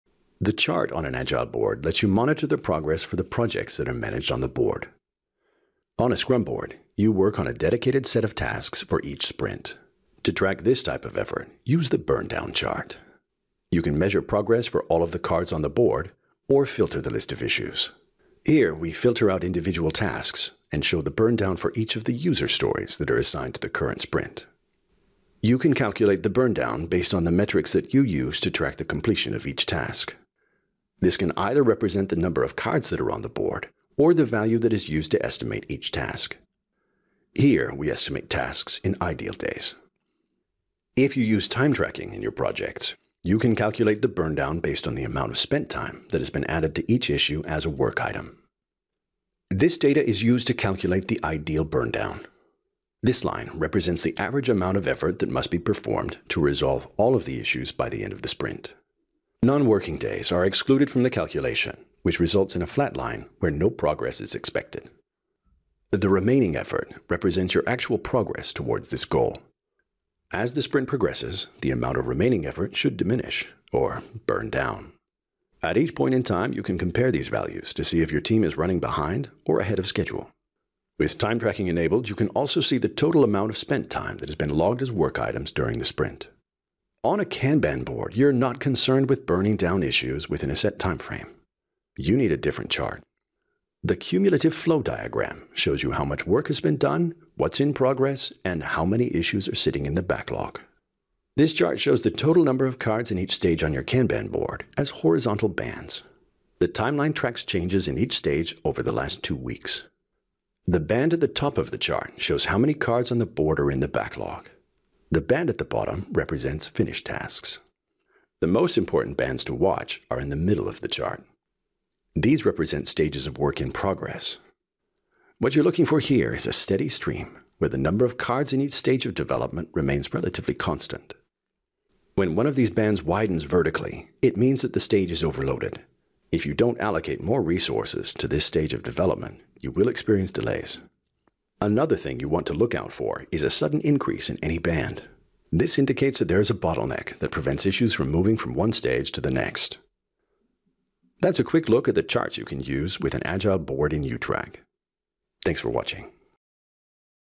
Chart Options for Agile Boards
This demo describes the Burndown and Cumulative flow charts on the Agile board in YouTrack. These charts help you to monitor your team progress at a glance.